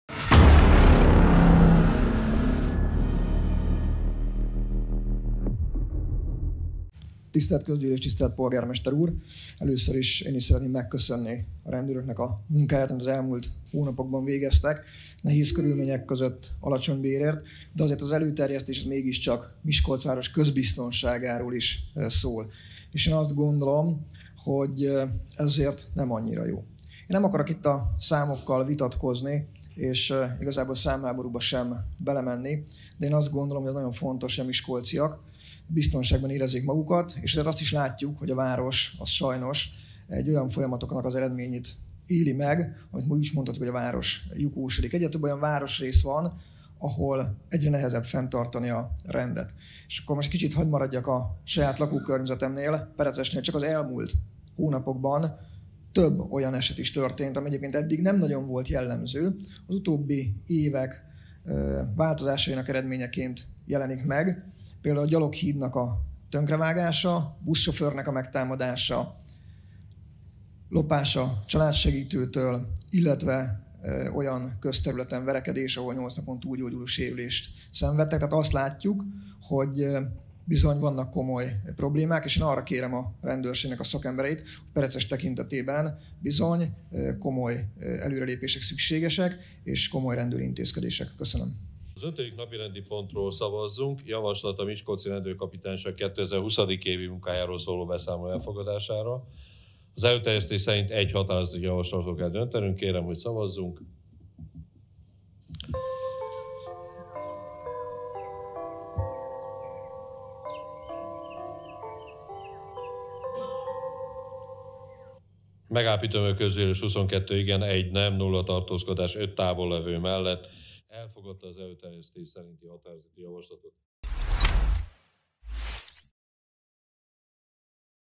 Csak a Mi Hazánk nem támogatta a Miskolc közbiztonságáról szóló rendőrségi beszámoló elfogadását
Rend van Miskolcon?
Csak a Mi Hazánk nem támogatta a város közbiztonságáról szóló rendőrségi beszámoló elfogadását.
A  legutóbbi közgyűlésen Fidesz és a balliberális pártok (Jobbikkal szövetségben) együtt elfogadták a rendőrségi beszámolót Miskolcon (szavazás a videóban).
Pakusza Zoltán külön felhívta a rendőrség vezetőinek  figyelmét arra, hogy az utóbbi időben Perecesen is megszaporodtak a bűncselekmények.